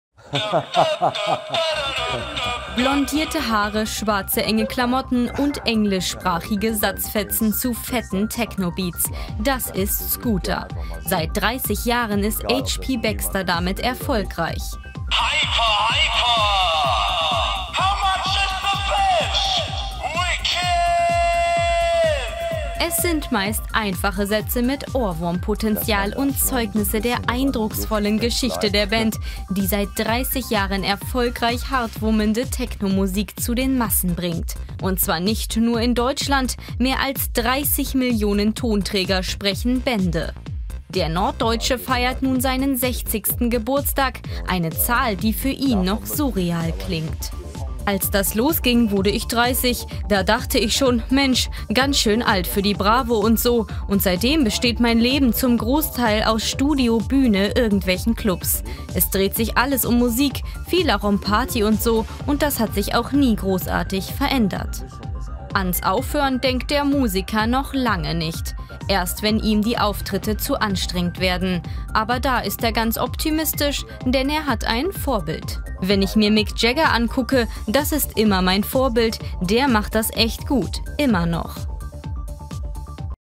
30 Jahre Scooter: Frontmann H.P. Baxxter sieht noch kein Ende
30 Jahre Scooter: Frontmann H.P. Baxxter sieht noch kein Ende

German/Deutsch

Blondierte Haare, schwarze enge Klamotten und englischsprachige Satzfetzen zu fetten Technobeats - das ist Scooter. Seit 30 Jahren ist H.P. Baxxter damit erfolgreich. Sein Vorbild ist Mick Jagger.